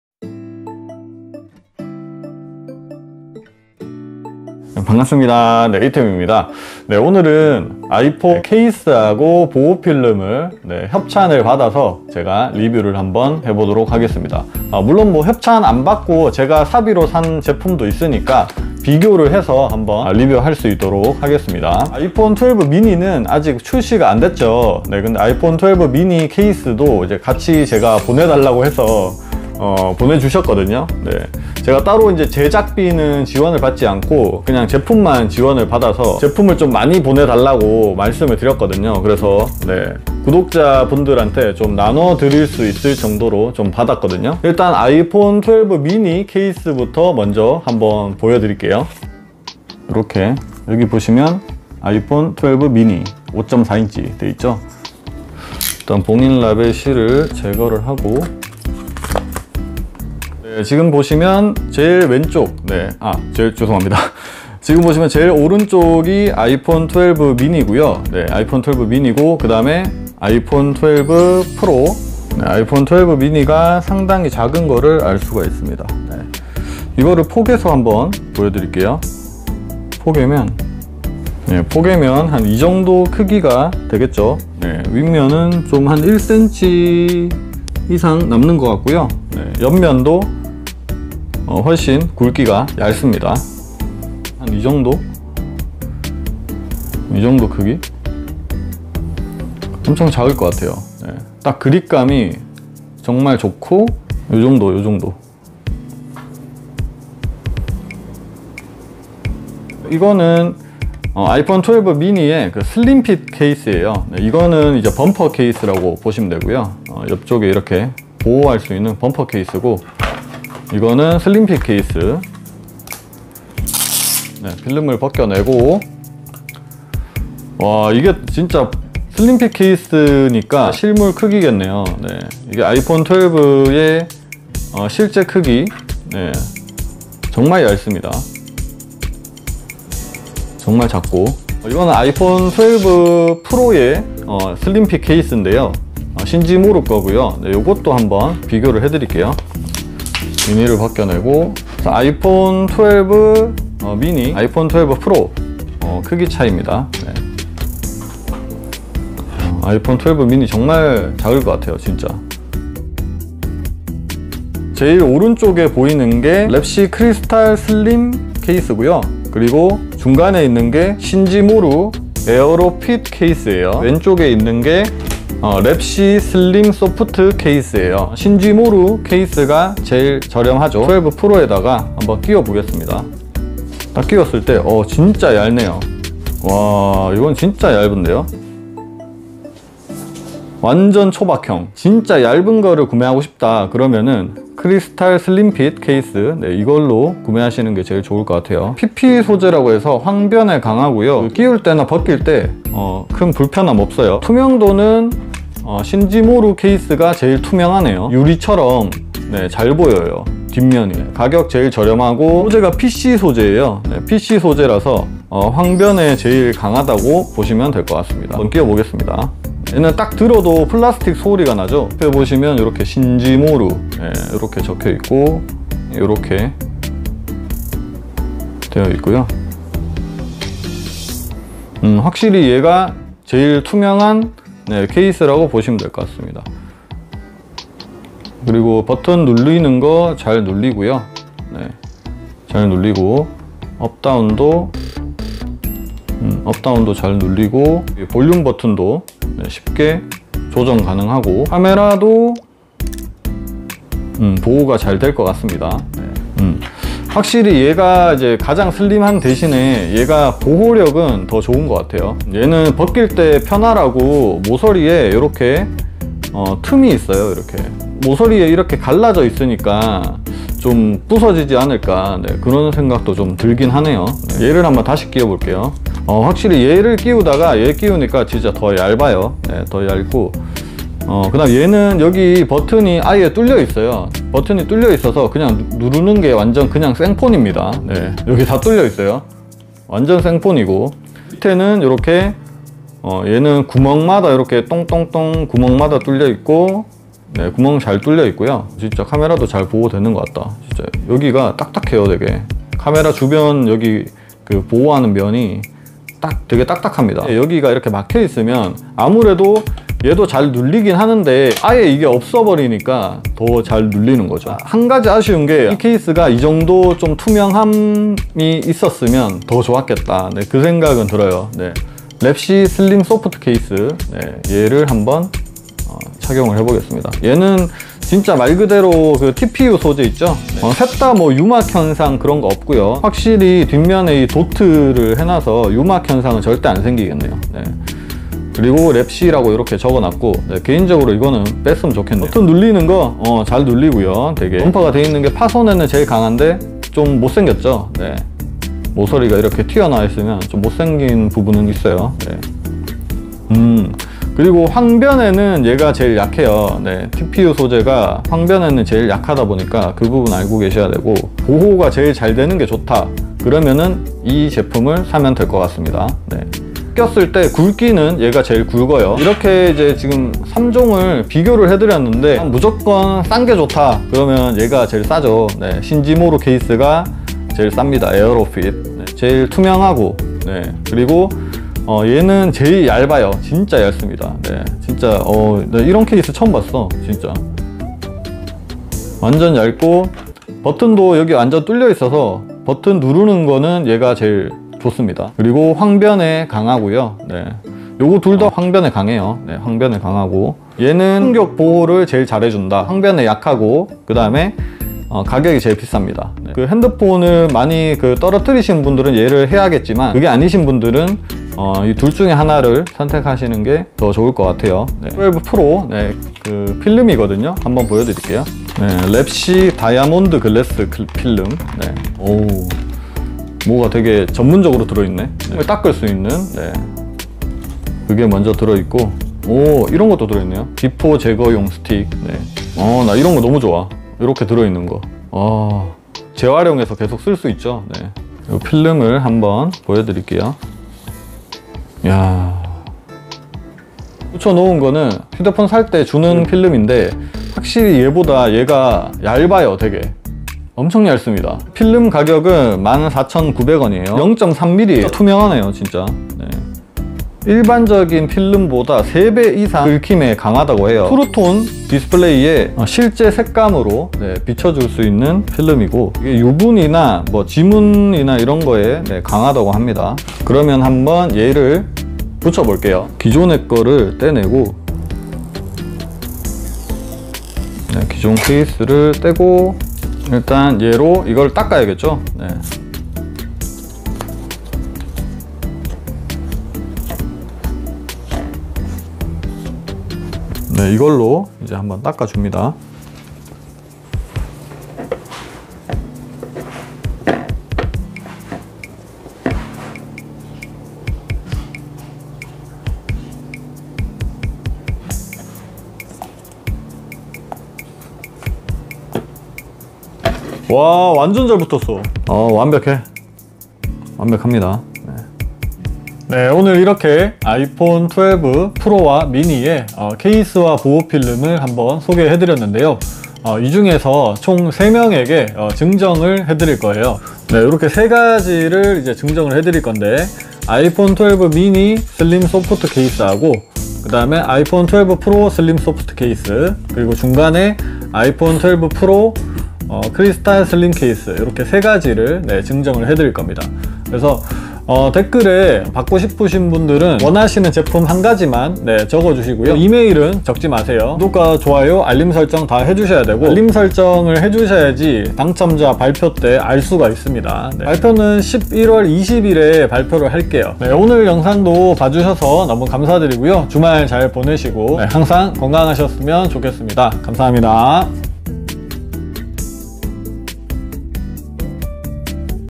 [구독자이벤트] 아이폰12케이스 3종 비교! 아이폰12 미니 케이스도 보여드릴게요^^
제품은 업체로부터 전달받았습니다.

1. 랩씨 갤럭시 아이폰 기종별 크리스탈 슬림핏 0.4 케이스

2. 랩씨 아이폰12/ 아이폰12 프로 슬림 소프트 투명 케이스

3. 랩씨 아이폰12/ 12프로 강화유리 액정보호필름